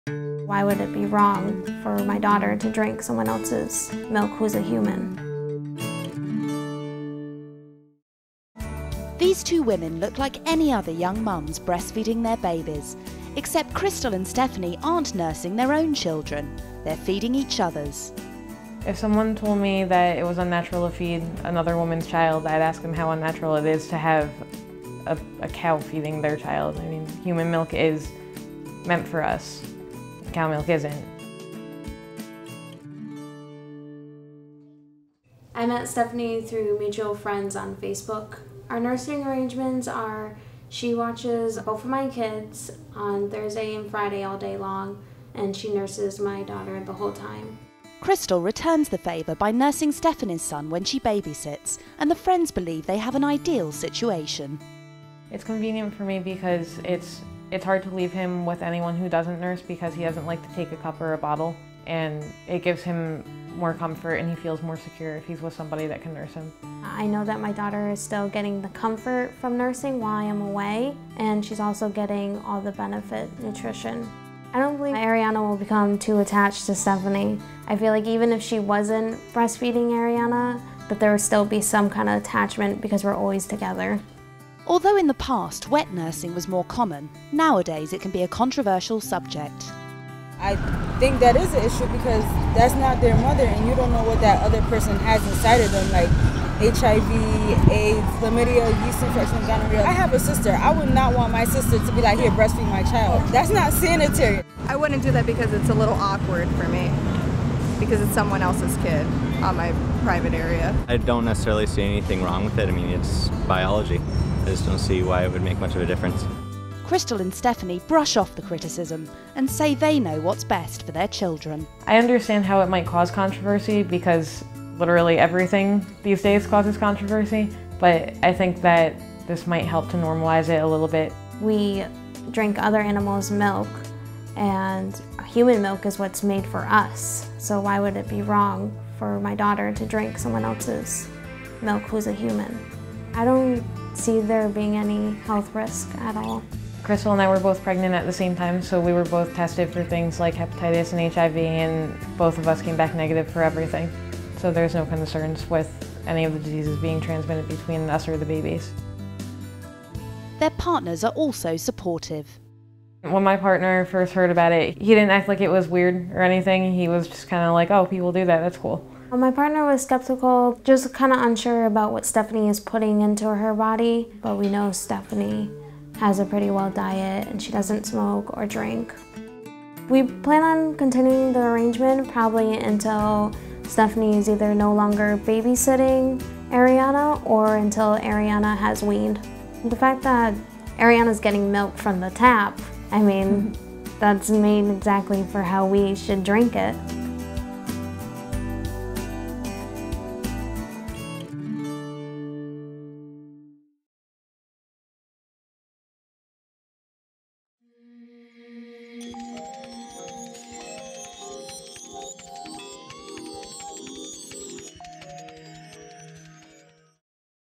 We Breastfeed Each Other's Kids
TWO young mums who breastfeed each other’s children are encouraging other women to share their milk. Stefani Tatavitto, 23, regularly feeds both her own two-year-old son and her friend Chrystal Klein’s 19-month-old daughter while Chrystal works at a factory. And when Stefani goes on a night out, Chrystal, also 23, returns the favour – nursing both tots as if they were her own. The friends – who met on Facebook five years ago – started their unusual arrangement when Chrystal’s daughter, Ariana, was just ten months old.

Videographer / Director: Ruaridh Connellan
Producer: Emma Pearson
Editor: Sonia Estal